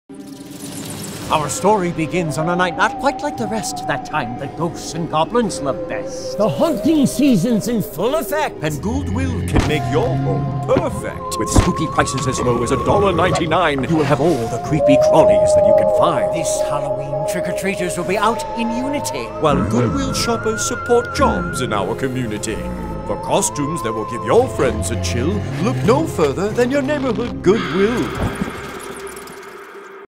Goodwill-Easter Seals Minnesota Halloween 2022
Get spooktacular Halloween savings at your neighborhood Goodwill. Save big on fall home décor, lights, creative costumes, cozy sweaters and everything in between to welcome fall and make your Halloween howl. Prices start at just ninety-nine cents, with thousands of items added daily.
Make Halloween magic with low prices and one-of-a-kind items at your neighborhood Goodwill. Visit GESMN.org to find your nearest store.

Your donations & purchases support local job training & placement programs for our participants. Learn about what we do for our fellow Minnesotans